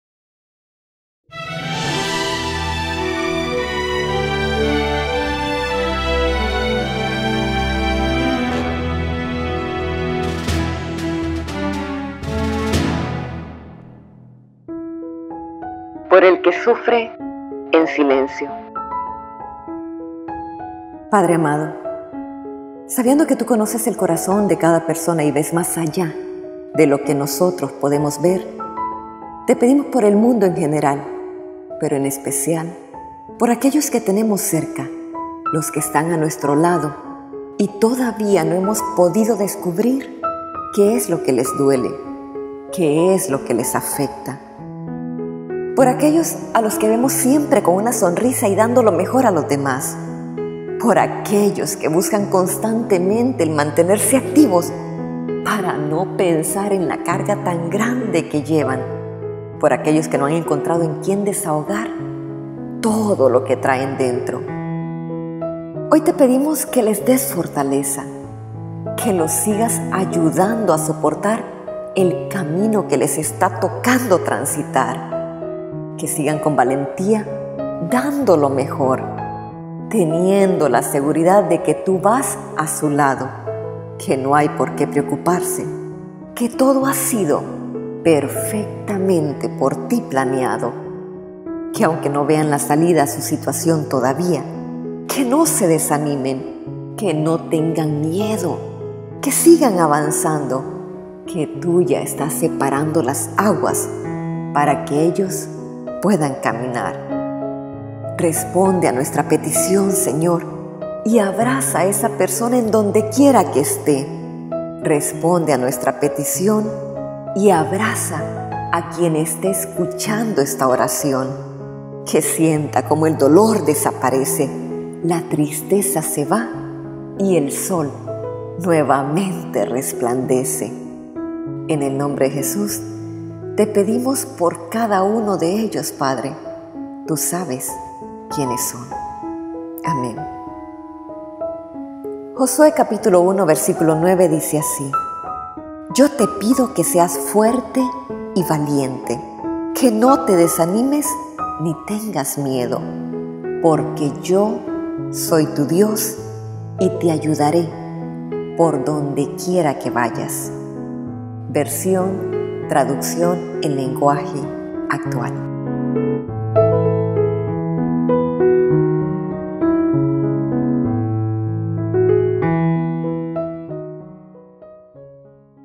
ORACIONES PARA REFLEXIONAR "POR EL QUE SUFRE EN SILENCIO"
POR EL QUE SUFRE EN SILENCIO

Padre amado, sabiendo que Tú conoces el corazón de cada persona y ves más allá, de lo que nosotros podemos ver, te pedimos por el mundo en general, pero en especial, por aquellos que tenemos cerca, los que están a nuestro lado y todavía no hemos podido descubrir, que es lo que les duele... que es lo que les afecta.
Por aquellos a los que vemos siempre con una sonrisa y dando lo mejor a los demás, por aquellos que buscan constantemente el mantenerse activos, para no pensar en la carga tan grande que llevan, por aquellos que no han encontrado en quien desahogar, todo lo que traen dentro.
Hoy te pedimos que les des fortaleza, que los sigas ayudando a soportar, el camino que les está tocando transitar. Que sigan con valentía dando lo mejor, teniendo la seguridad de que Tú vas a su lado, que no hay por qué preocuparse, que todo ha sido, perfectamente por Ti planeado.
Que aunque no vean la salida a su situación todavía, que no se desanimen, que no tengan miedo, que sigan avanzando, que Tú ya estás separando las aguas, para que ellos puedan caminar.
Responde a nuestra petición Señor y abraza a esa persona en donde quiera que esté, responde a nuestra petición y abraza a quien esté leyendo esta oración, que sienta como el dolor se desaparece, la tristeza se va y el sol nuevamente resplandece.
En el nombre de Jesús te pedimos por cada uno de ellos Padre, Tú sabes quienes son.
Amén.
Josue 1:9 (TLA) »Yo te pido que seas fuerte y valiente, que no te desanimes ni tengas miedo, porque yo soy tu Dios, y te ayudaré por dondequiera que vayas.